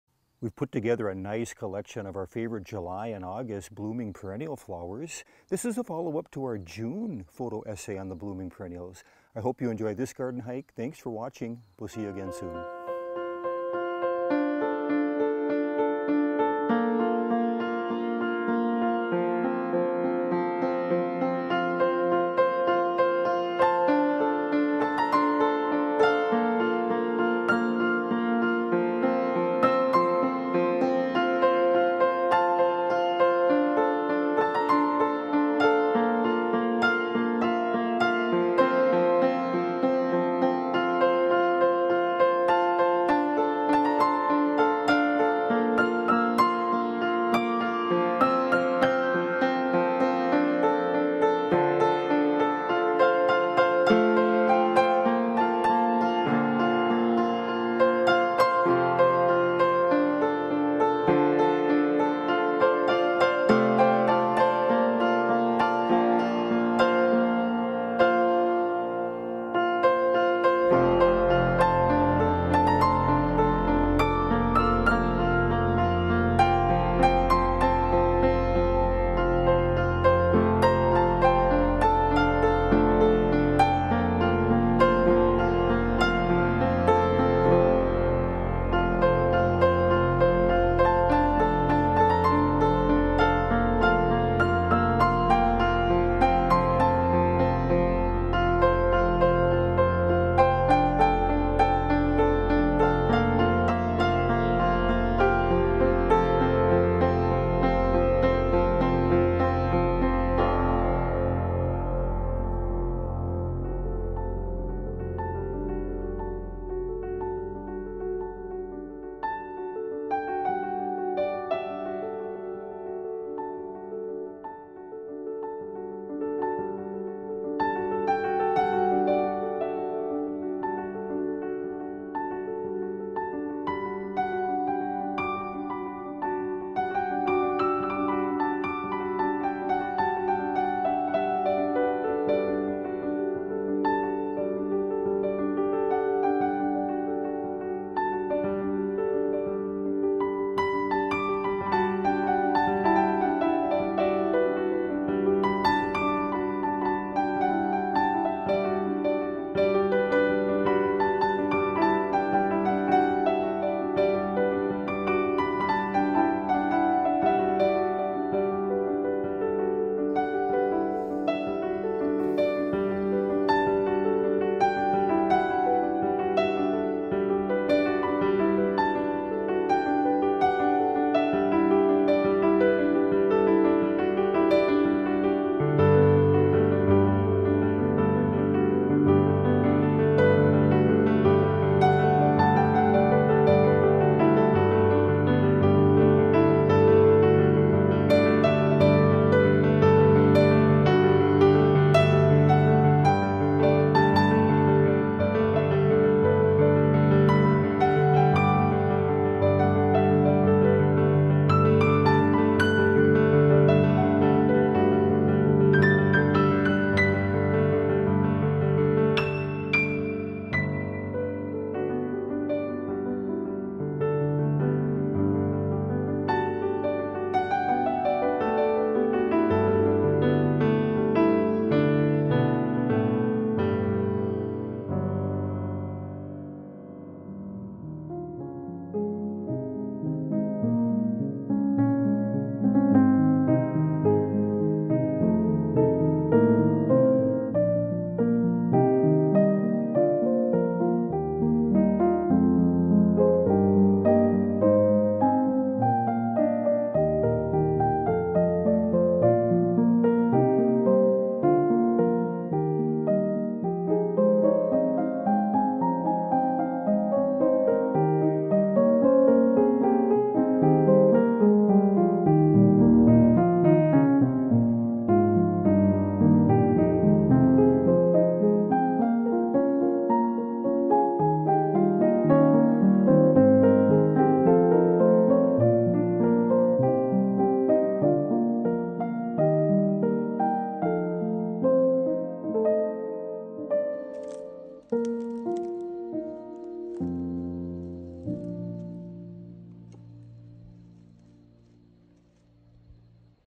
Great Blooming Perennials For Mid Summer - A Photo Essay
We put together a collection of some of the best-blooming perennials for July and August. This video is a photo essay highlighting perennials hardy to zone 3 & 4. From Coneflowers to Rudbeckias to Sedum, you're sure to find something you love.